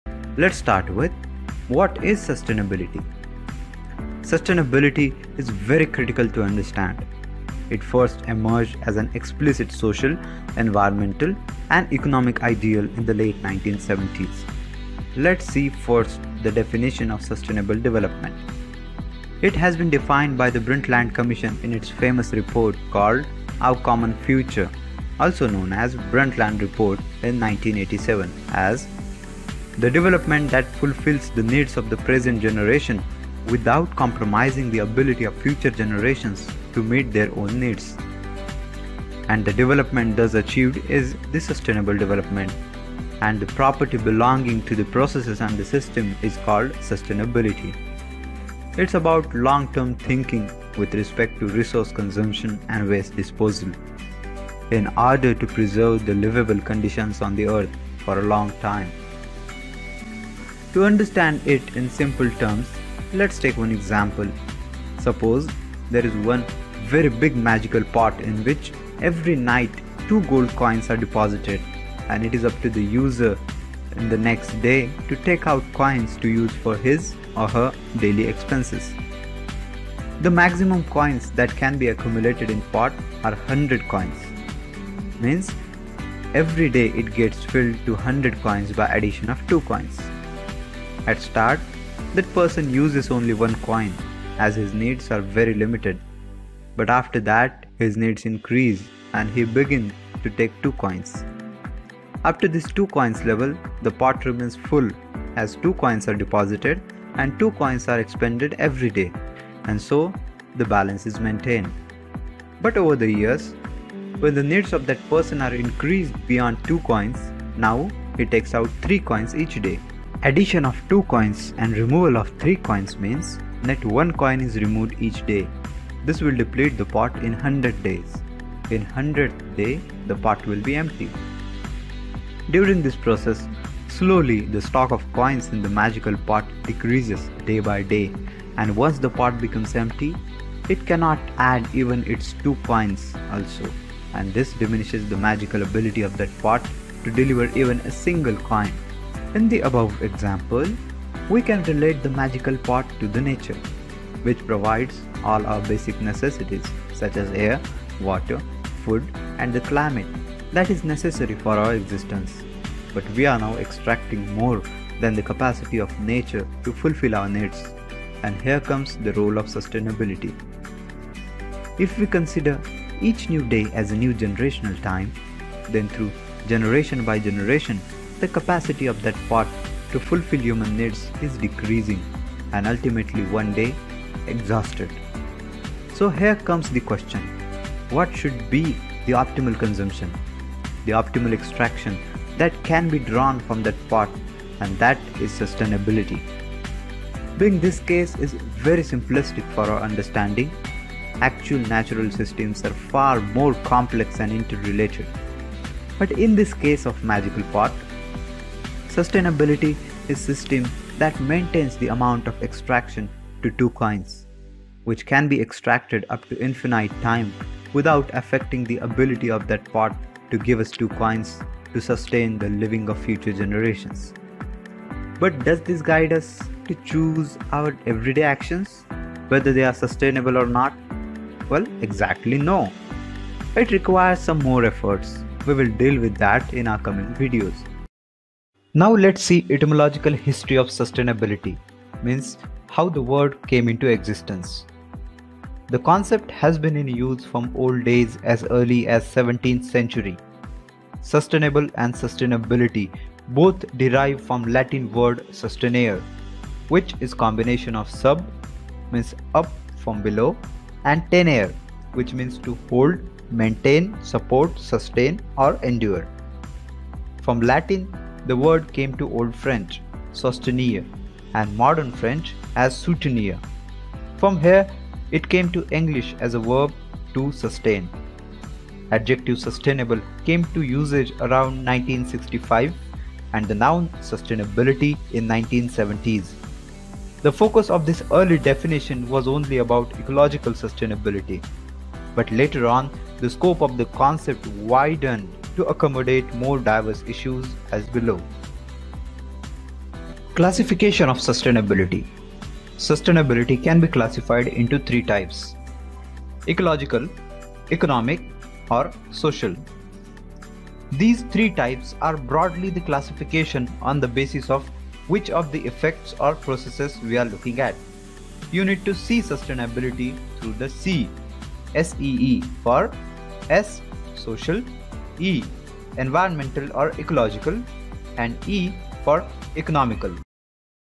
What is sustainability and Sustainable Development?
In this Sustainability video, we will try to understand
0:00 Concept of sustainability with an example of a Magical Pot
3:58  It’s Origin and Roots
5:00 Types of sustainability or 3 pillars of it

What is Sustainability?
Sustainable Development has been defined by the Brundtland Commission in its famous report called ‘Our Common Future’ also Brundtland Report in 1987 as “the development that fulfills the needs of the present generation without compromising the ability of future generations to meet their own needs.”

To understand it in simple terms, Let's take one example, there is one very big magical pot in which every night 2 gold coins are deposited and it is up to the user the next day to take out coins to use for his or her daily expenses.
But, over the years, when the needs of that person are increased beyond 2 coins, now he takes out 3 coins each day.

During this process, slowly the stock of the coins in that magical pot decreases day by day and once the pot becomes empty, it cannot add even its 2 coins also and this diminishes the magical ability of that pot to deliver even a single coin.

In the above example, we can relate the magical pot to the nature, which provides all our basic necessities, such as, air, water, food and the climate that is necessary for our existence. But we are now extracting more than the capacity of nature to fulfil our needs and here comes the role of sustainability.

Etymological history of sustainability:

Sustainable and Sustainability both derive from Latin word sustin¯ere, which is combination of sub –means ‘up form below’ and ‘ten¯ere’- which means ‘to hold, maintain, support, sustain, or endure’.

Learn the basics of Sustainability with me through the course on Udemy: ‘Basics of Sustainability: Learn in Simple Language'

Reference and Source:
1. Report of the World Commission on Environment and Development: Our Common Future, p.n. 15
2. Etymological History from Book ‘Sustainability A History’, Jaremy Caradonna
3. Image of ThreePillars_ArchitecturalSimple-Thwink.org